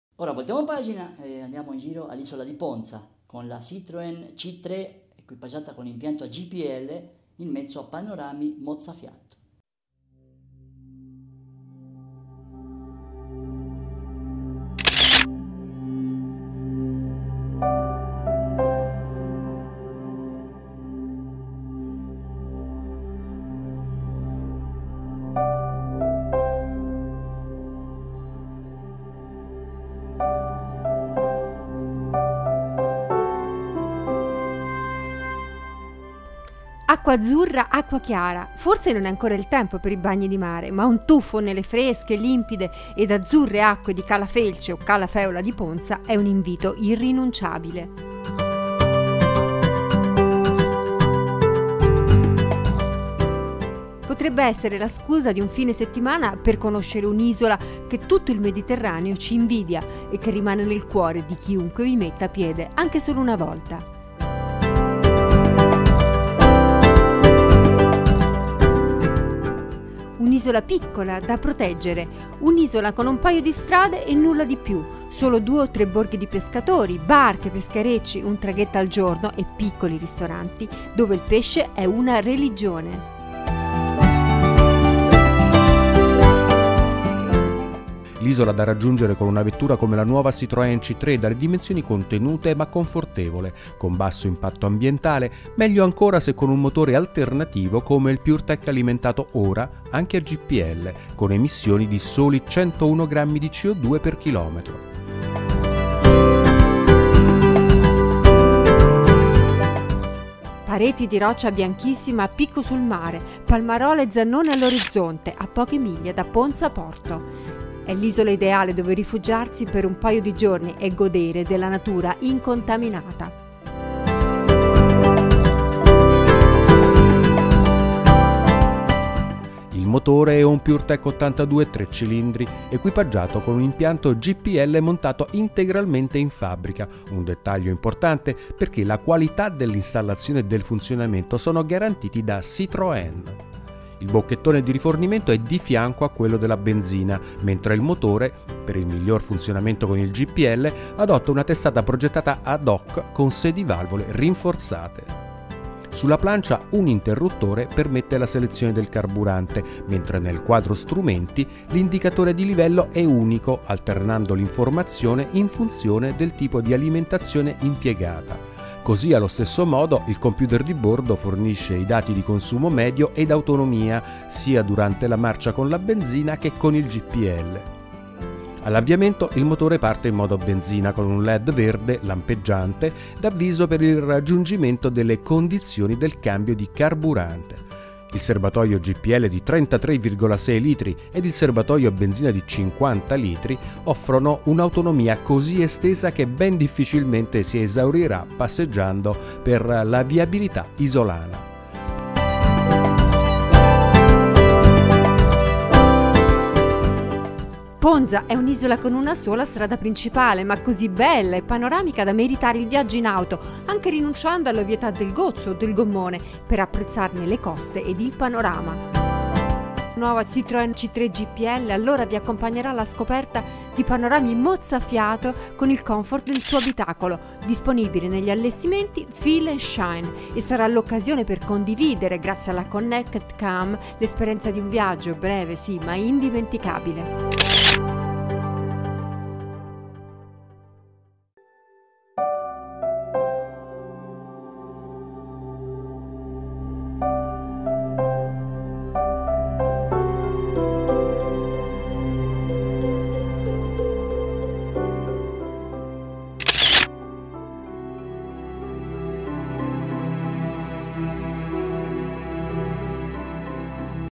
All’Isola di Ponza con la Nuova Citroën C3 a GPL - Electric Motor News n° 19 (2017)
L’isola di Ponza è da raggiungere con una vettura come la nuova Citroën C3, dalle dimensioni contenute ma confortevole, con basso impatto ambientale, meglio ancora se con un motore “alternativo” come il Pure Tech alimentato ora anche a GPL con emissioni di soli 101 grammi di CO2 per chilometro.